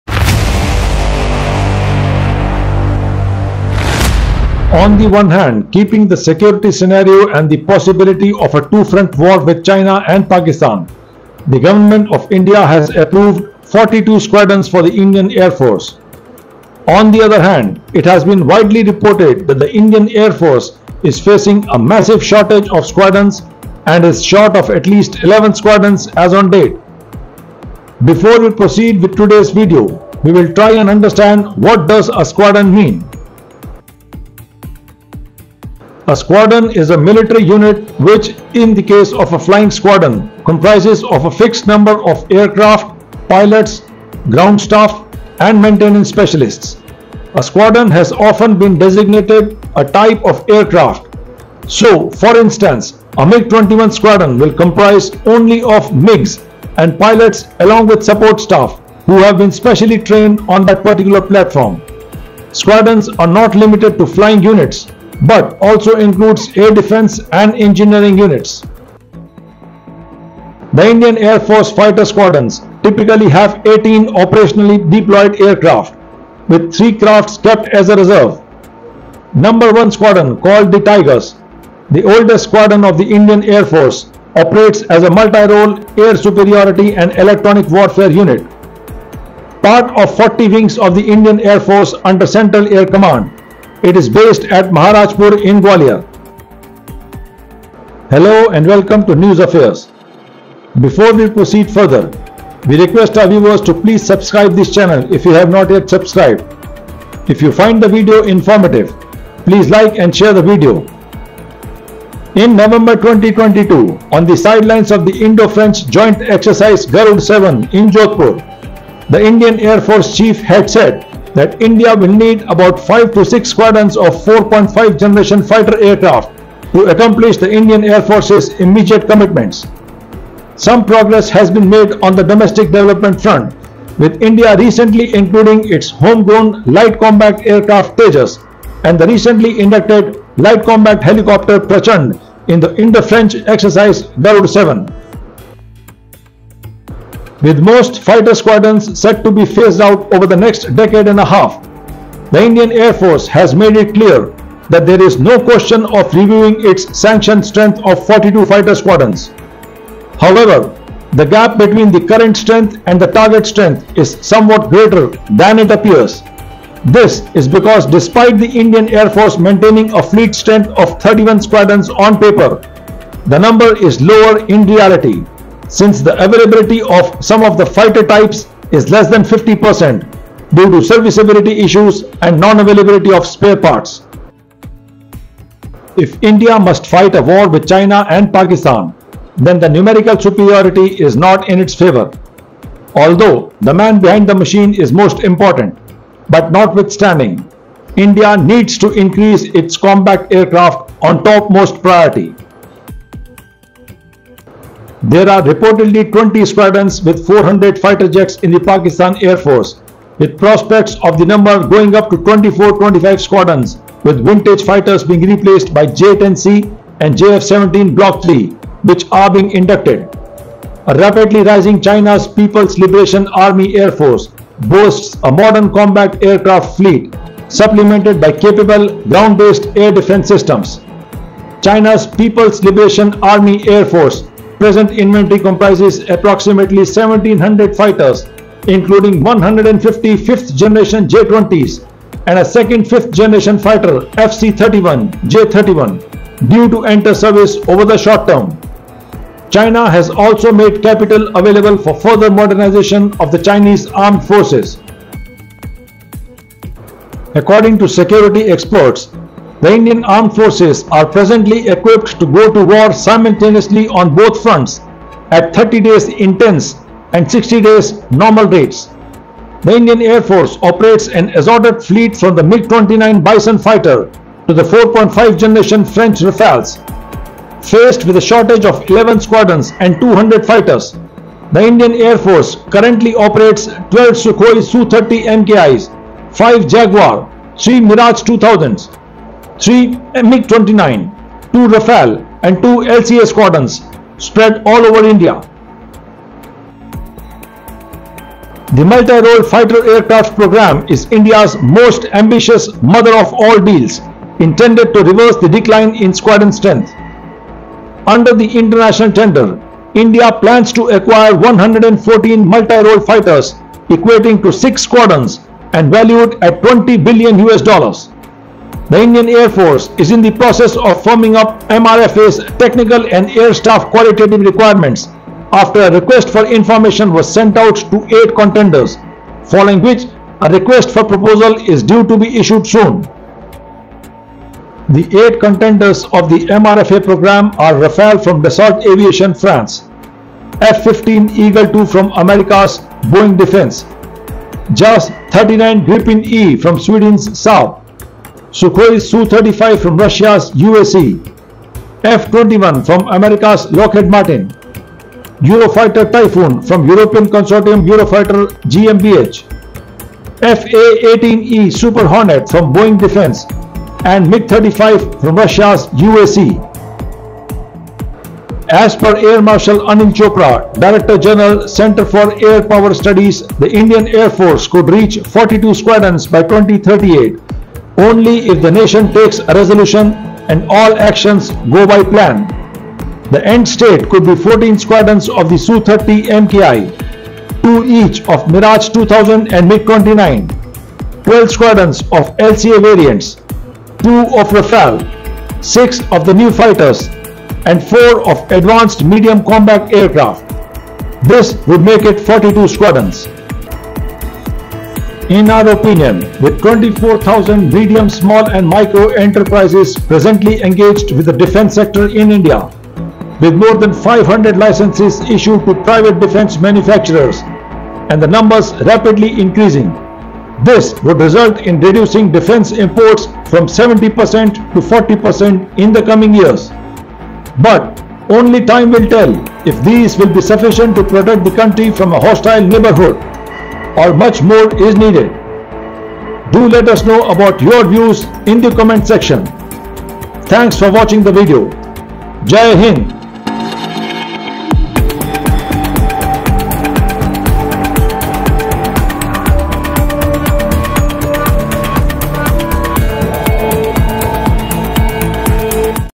What is a SQUADRON in INDIAN AIR FORCE? How many planes and pilots?
WHAT IS A SQUADRON IN THE INDIAN AIR FORCE?

एक स्क्वाड्रन में कितने प्लेन होते हैं?
How many planes are in one squadron?

What is the difference between wing and squadron in IAF?

In this informative video, we delve into the concept of a squadron within the Indian Air Force. Discover the structure, roles, and significance of squadrons in maintaining aerial defense and operational readiness. Join us as we explore the various types of squadrons, their functions, and the vital contributions they make to national security.